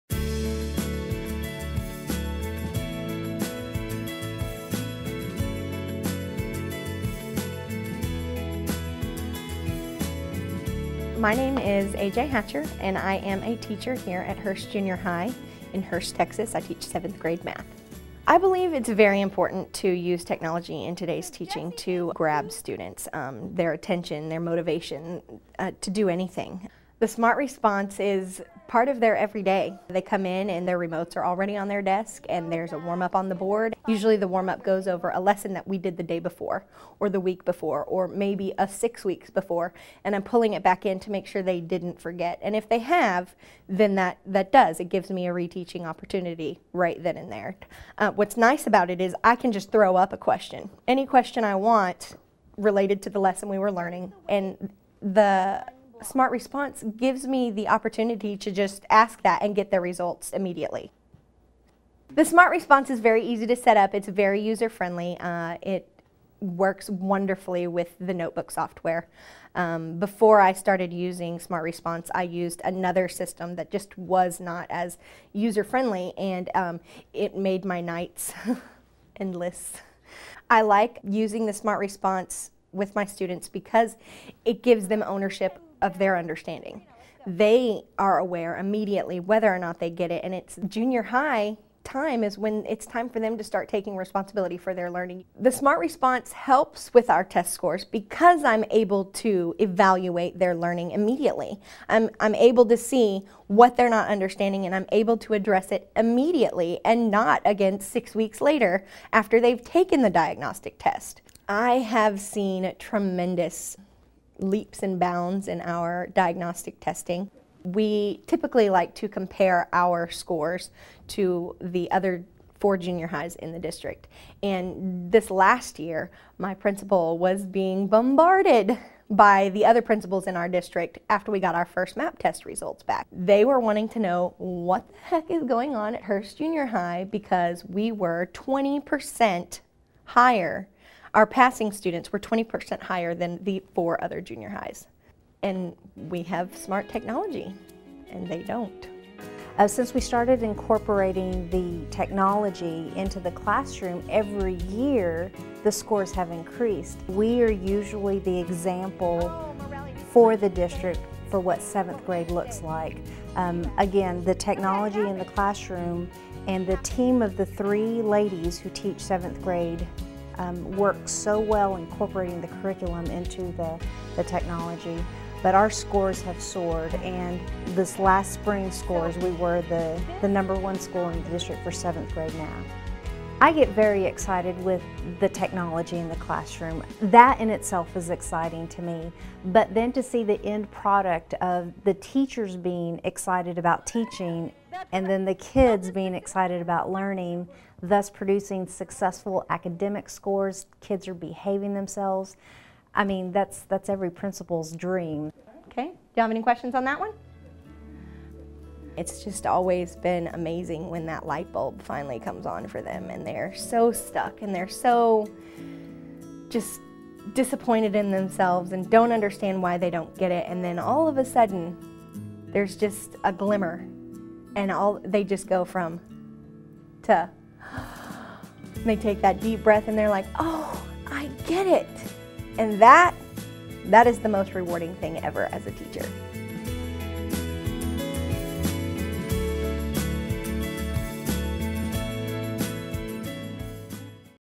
Math Scores Soar with SMART Response
TeacherTube User: SMARTClassrooms

Discover how using the SMART Response interactive response system helped Hurst Junior High in Texas become the number one school in its district for seventh grade math. SMART Response helps with our test scores because Im able to evaluate their learning immediately say